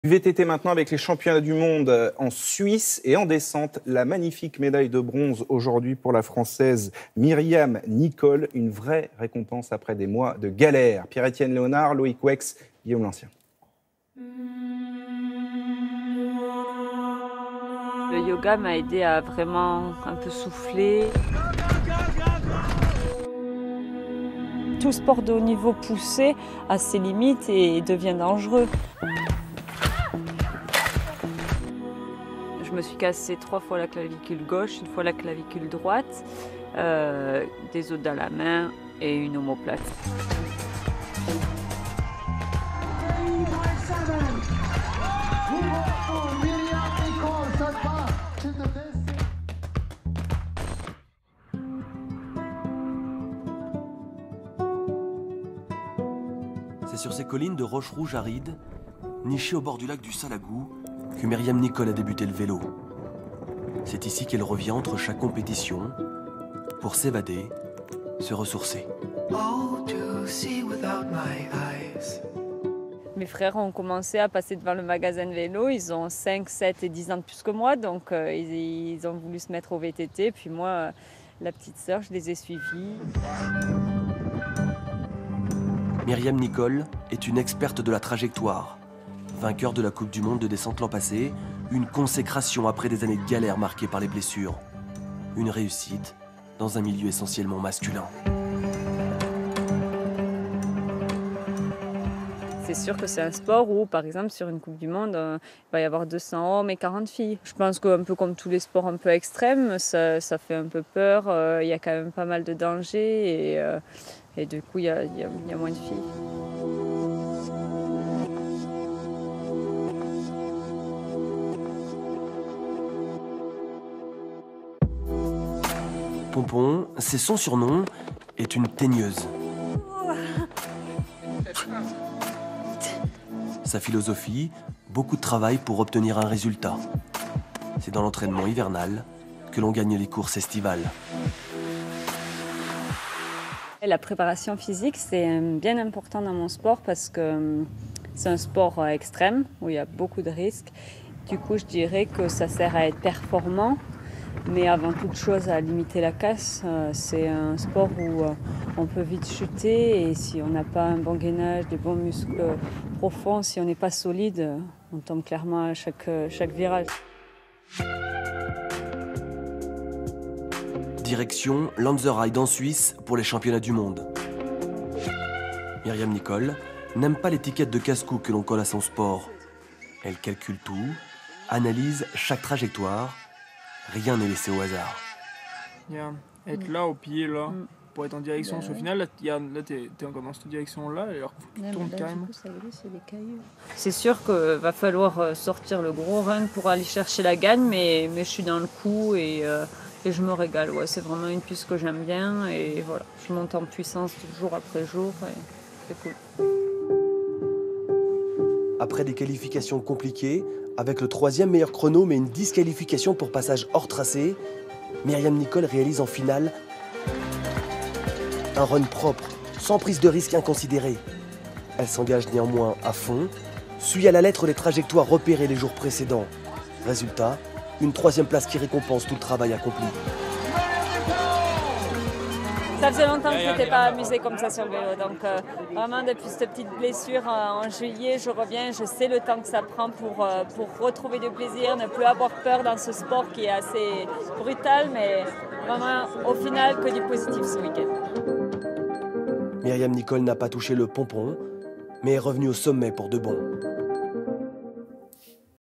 Myriam Nicole, de la descente aux enfers au retour au sommet
Qui dit VTT, dit aussi (souvent) belles gamelles et sacrées frayeurs. Myriam Nicole en sait quelque chose. La Française a connu les pépins, les blessures avant de revenir à son meilleur niveau. Celui qui a fait d'elle la numéro un mondial dans sa discipline et une multiple médaillée en championnats du monde. La Montpelliéraine de 28 ans a récidivé ce dimanche en décrochant le bronze. Stade 2 vous brosse le portrait de "Ponpon" la championne.